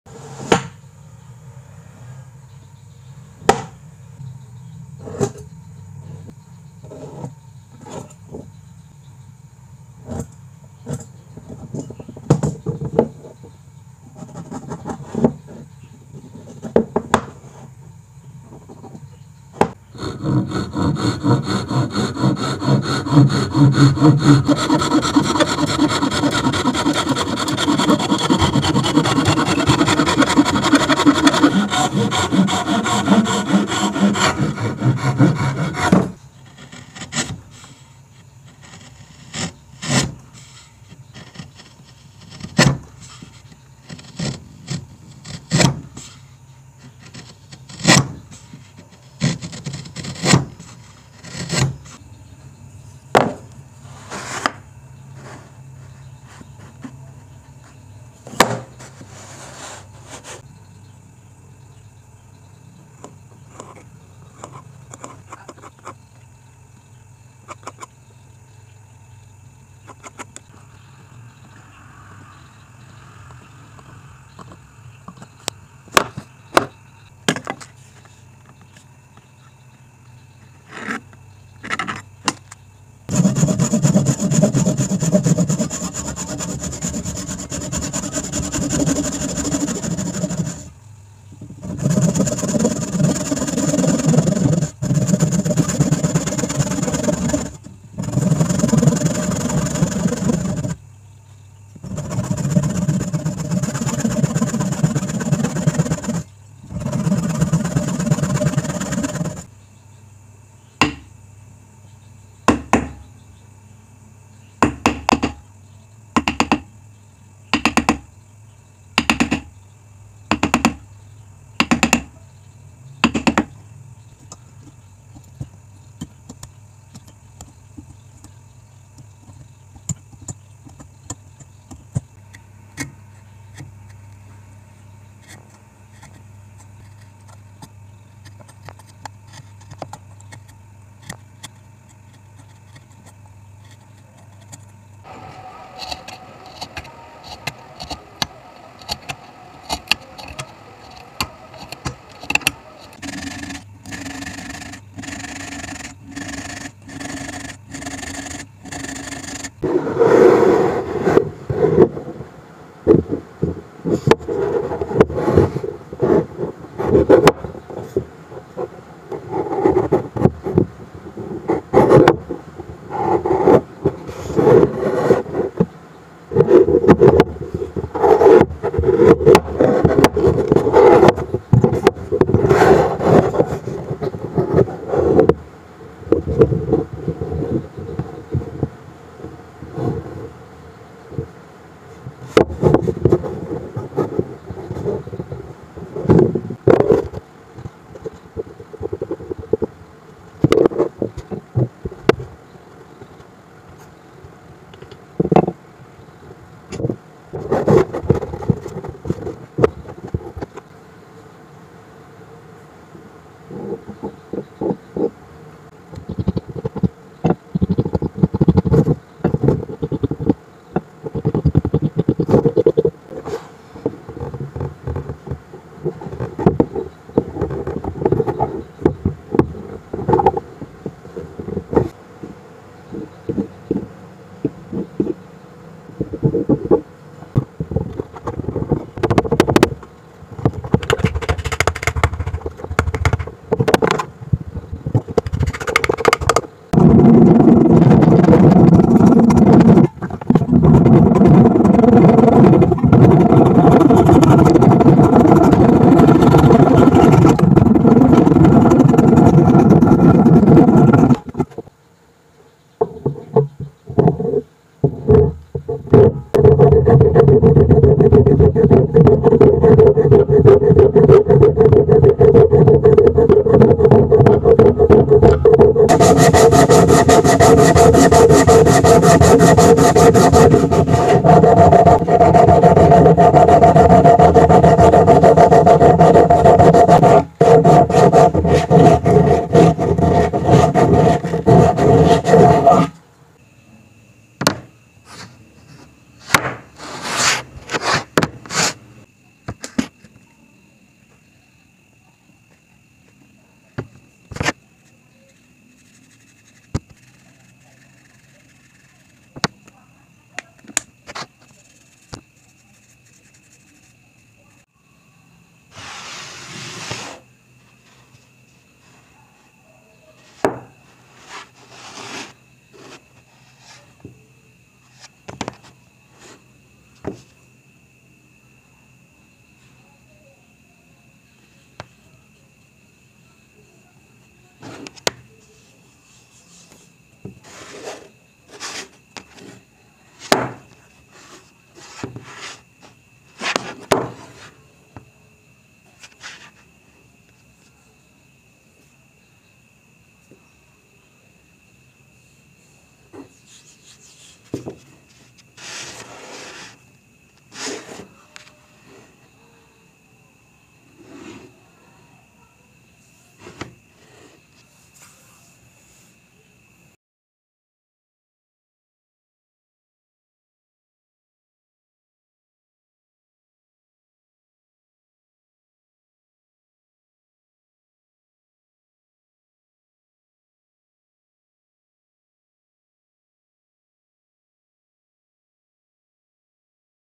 Rain storm technique wood connection / Amazing Japanese Woodworking
Amazing Woodworking Techniques that Japanese carpenter do in Joint wood.
In making construction, there are those who prioritize beauty and maximize strength, namely the combination of art with the skill of carpenter in creating extraordinary things.
This wood joint technique is a carpenter technique in Japanese.
This is an amazing rainstorm wood joining technique that sticks to the corner joints of the wood.
I will show you how the wood connection technique in Japan is done using a chisel.
This technique is great for wooden corner meetings .
I work in the traditional way .
I did this work with a tool you will know is incredibly easy to understand .